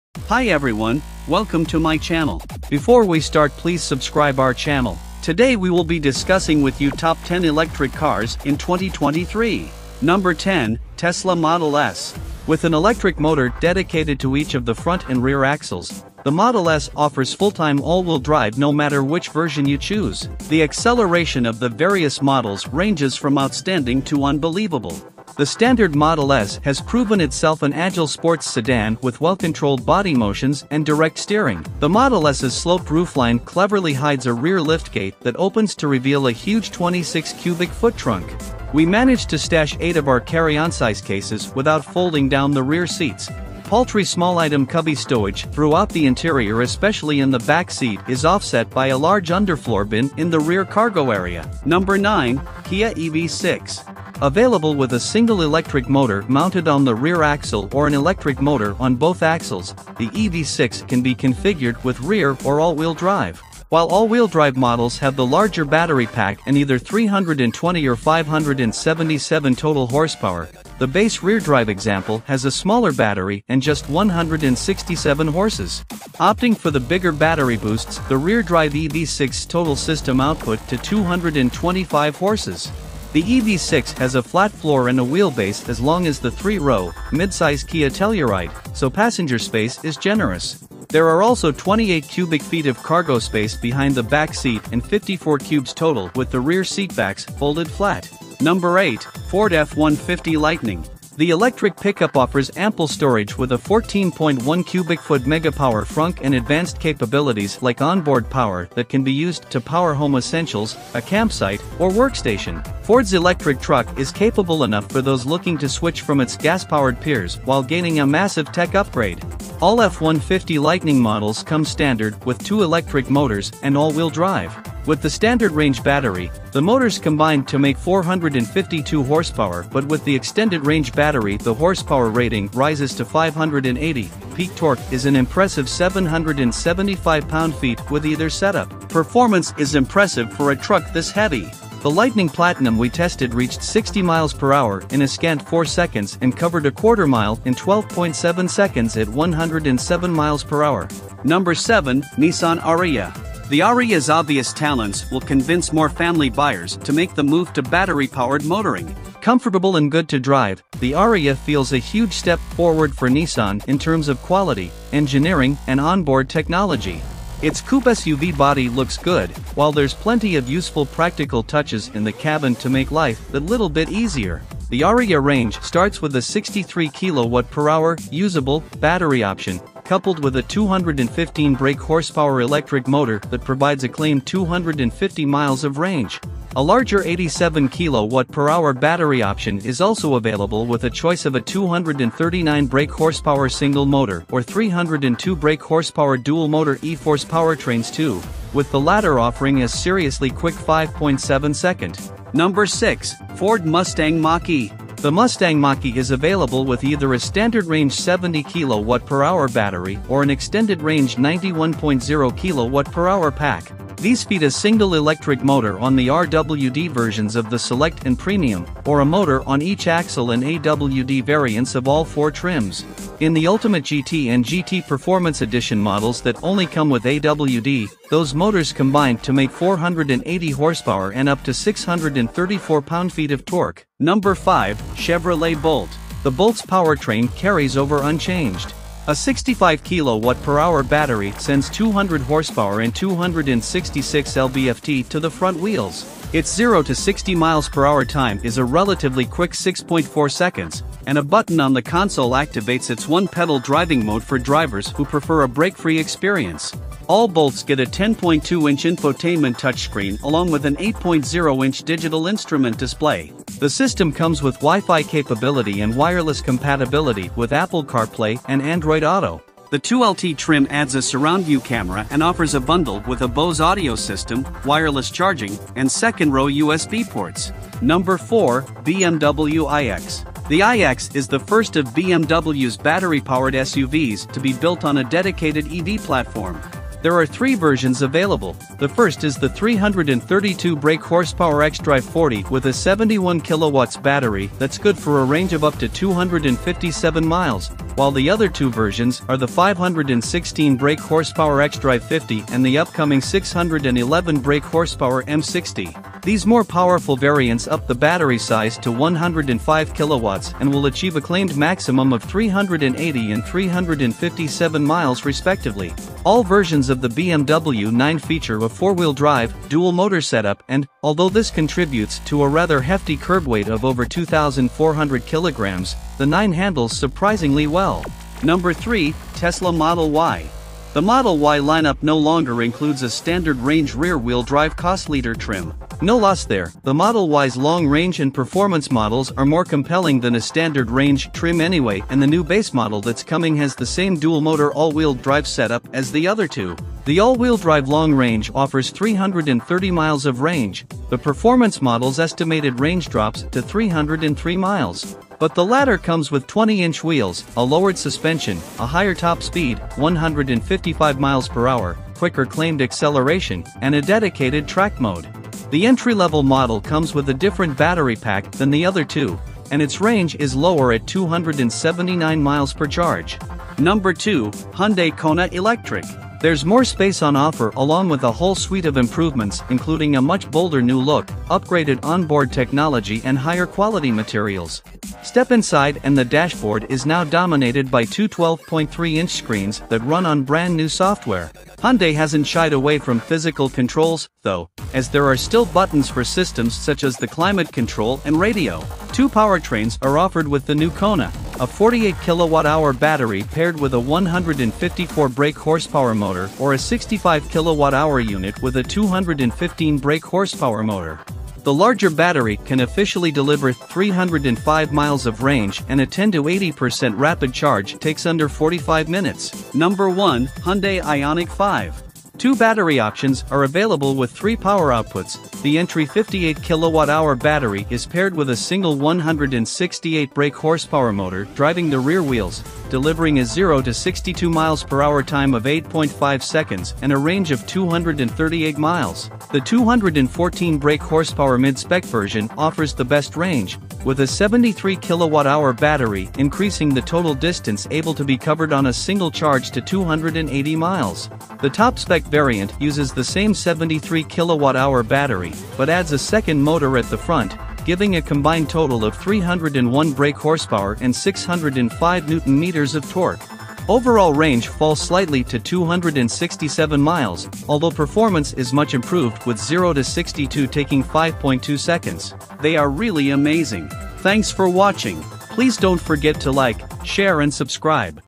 Top 10 Electric Cars in the World. Is Tesla the Best?
Overall, electric cars are a promising alternative in the effort to create more environmentally friendly and sustainable transportation. With ongoing technological advancements and growing awareness of environmental issues, electric cars are likely to play an increasingly important role on our roads in the coming years.